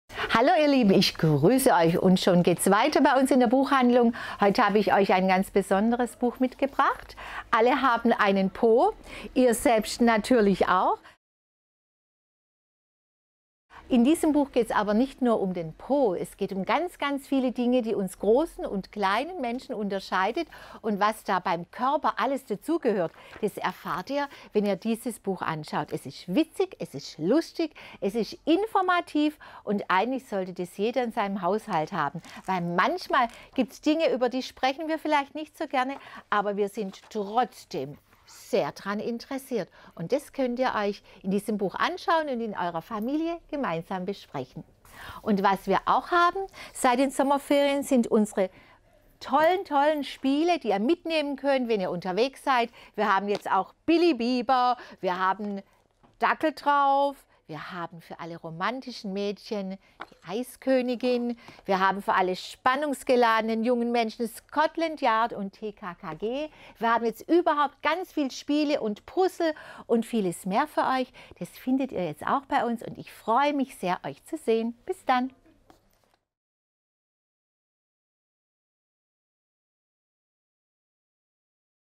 Alle haben einen Po | Soul Books - Susannes Bestseller und Buchtipps
"Alle haben einen Po" - ein wunderbares Buch, welches in jeden Haushalt gehört, finden wir :-)
Und ... NEU in eurer Buchhandlung am Markt: Jede Menge tolle SPIELE!
Kommt vorbei, schaut rein und spielt los - wir freuen uns auf euch!